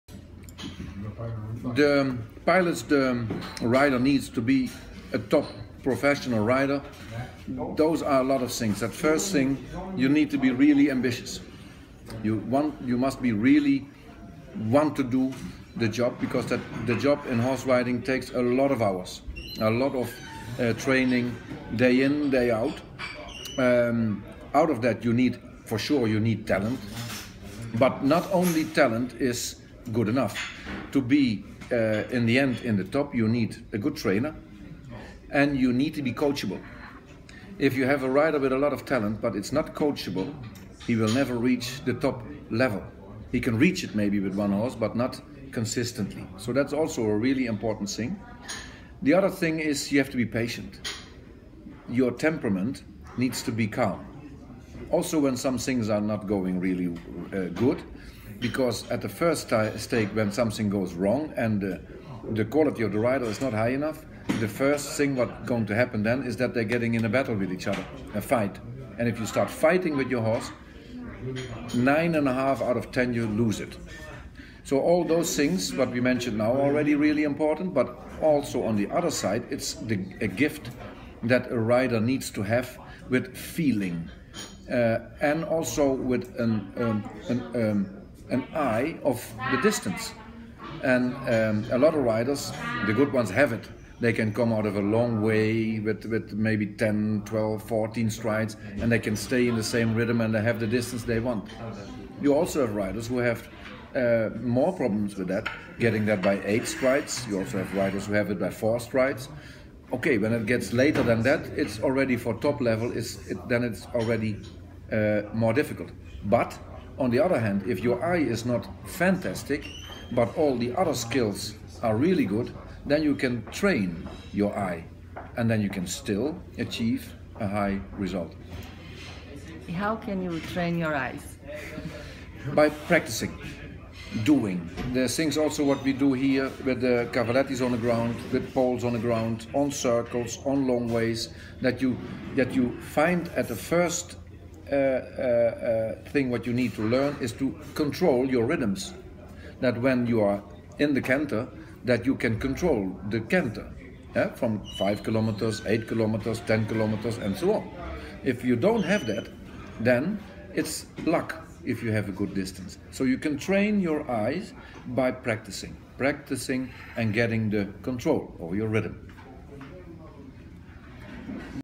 Rob Ehrens fala sobre as principais qualidades de um cavaleiro top!
Rob Ehrens, técnico da equipe Holandesa de salto fala sobre as principais qualidades de um cavaleiro top. Para ativar ou desativar a legenda em português, clique no retângulo ao lado da rodinha, assim que o vídeo for iniciado.